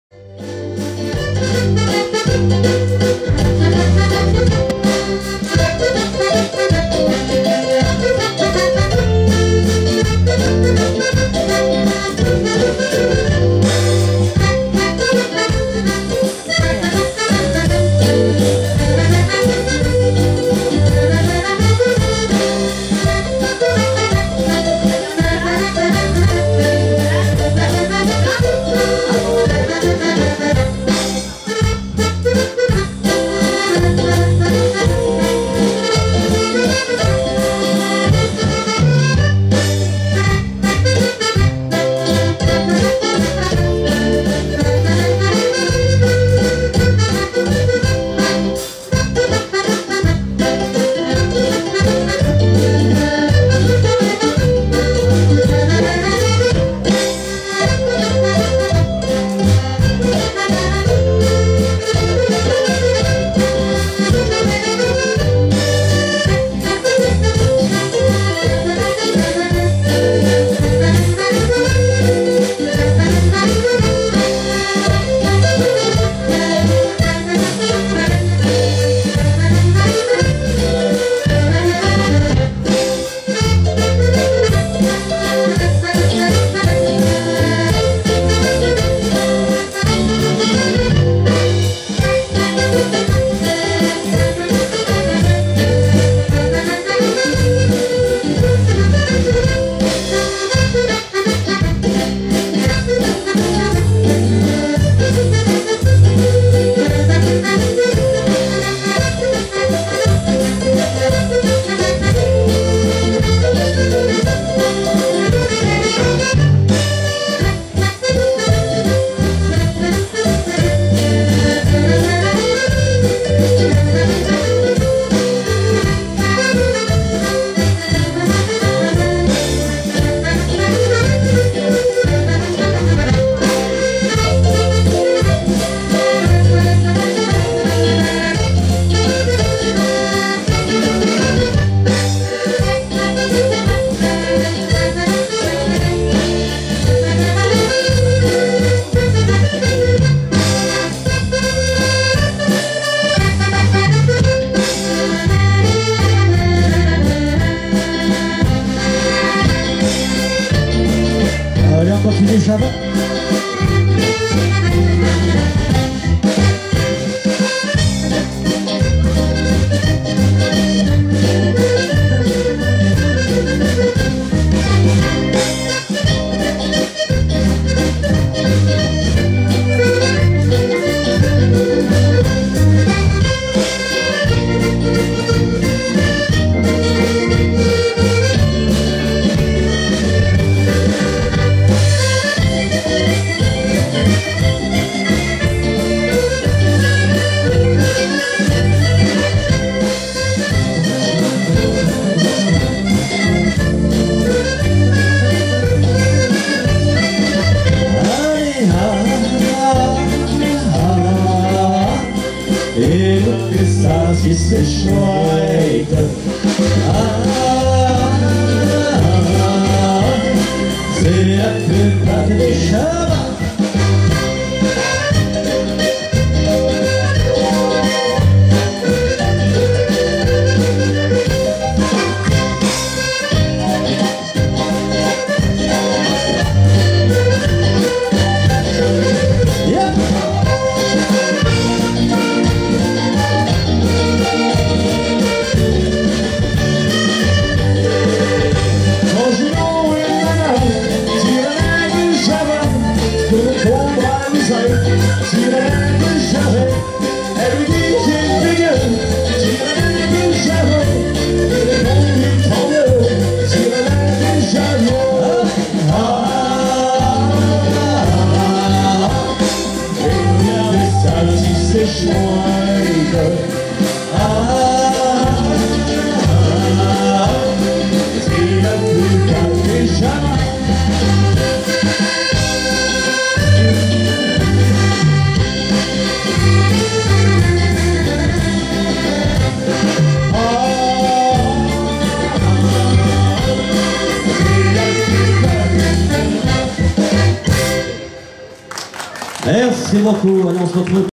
Manu PATRAS Objat aout 2016 Javas
Festival Chant'Accordéon OBJAT (19) 14 août 2016/ Orchestre de Fabien VEYRIRAS- vidéo et réalisation Alexandra Astorg (Sandy)